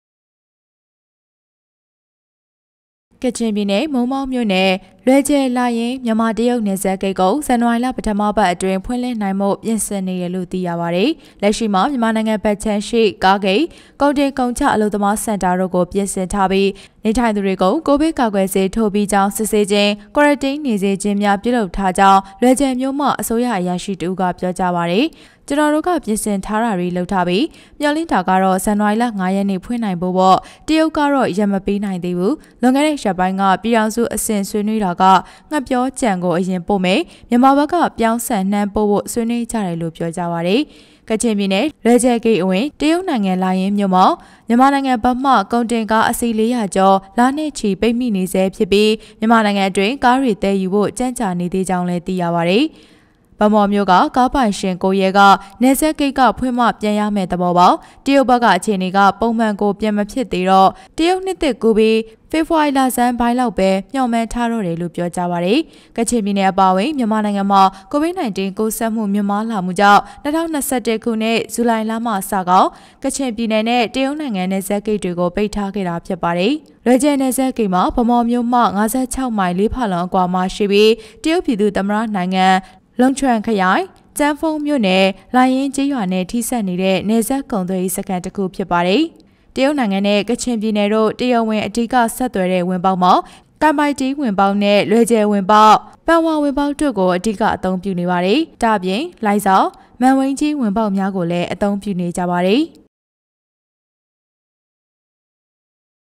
မြန်မာ-တရုတ်နယ်စပ်အဓိကဝင်ပေါက်တစ်ခုဖြစ်သည့် လွယ်ဂျယ်နယ်စပ်ဂိတ်ကို မကြာမီဖွင့်လှစ်နိုင်ရန် ပြင်ဆင်နေ
ကချင်ပြည်နယ်၊ မိုးမောက်မြို့နယ်၊လွယ်ဂျယ်-လာရင်းမြန်မာ-တရုတ် နယ်စပ်ဂိတ်ကို ဇန်နဝါရီပထမပတ်အတွင်း ဖွင့်လှစ်နိုင်ဖို့ ပြင်ဆင်နေတယ်လို့ သိရပါတယ်။